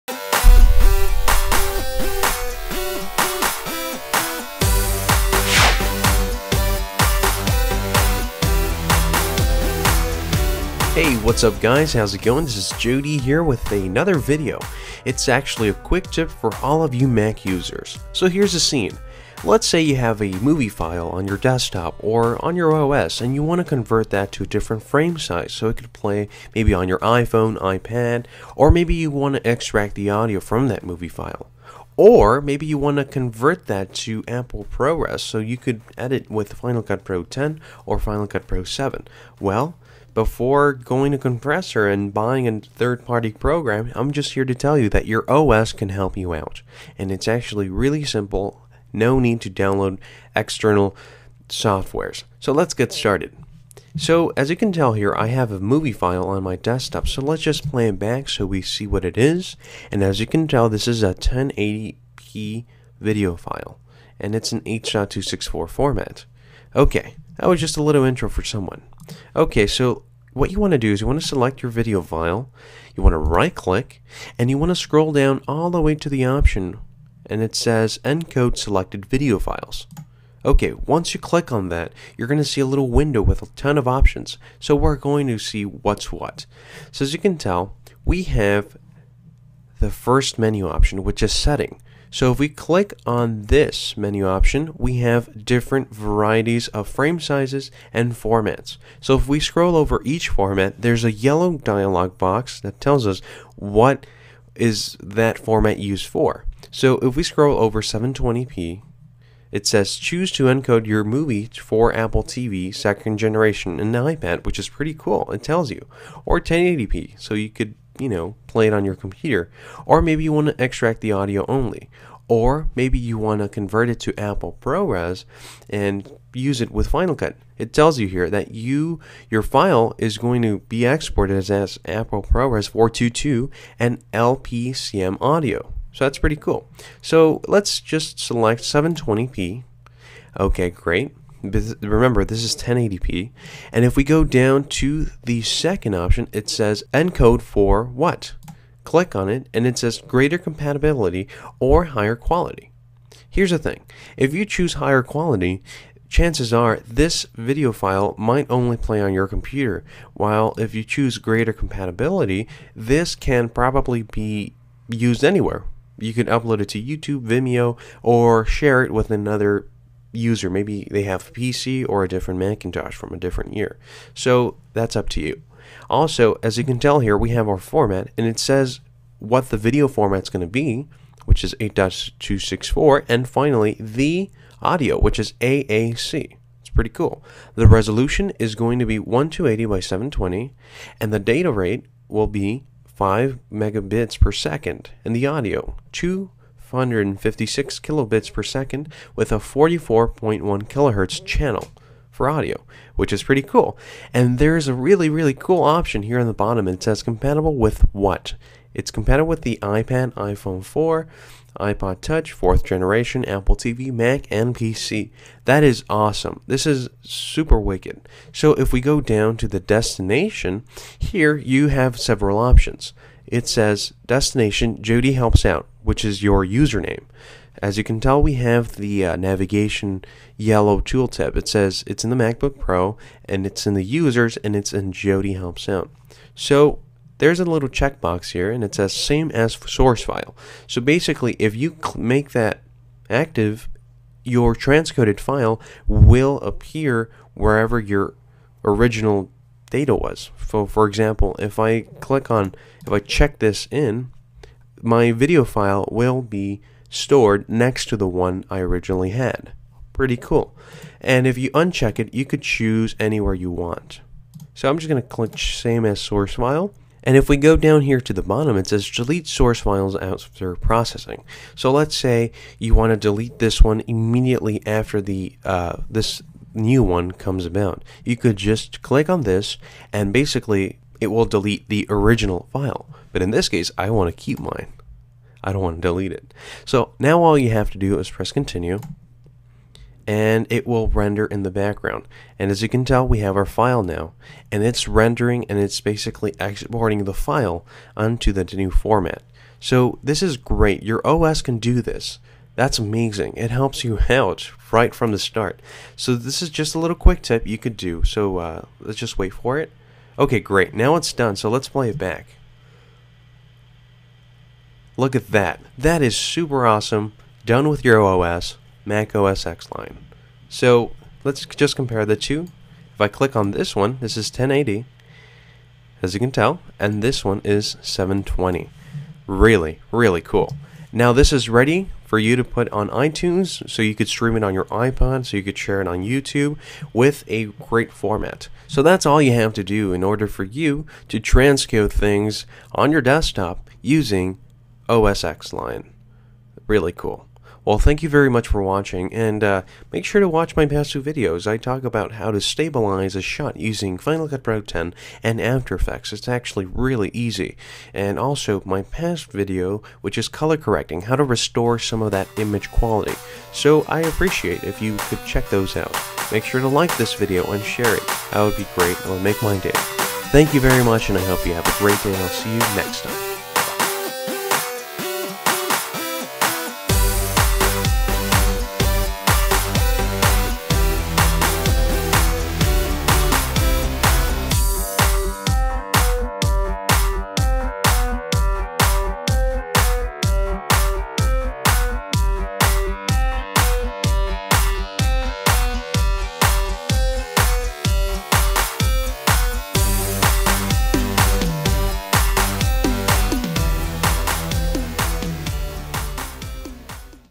Quicktip #9 Use Mac OS X to Encode your video files. (Really very cool!)
Hey guys I had to get this out, I just had to. This is a really cool feature Mac OS X Lion has, you can convert your videos to a format of your choice by using one menu option!

Incredible, amazing. No need for third party software!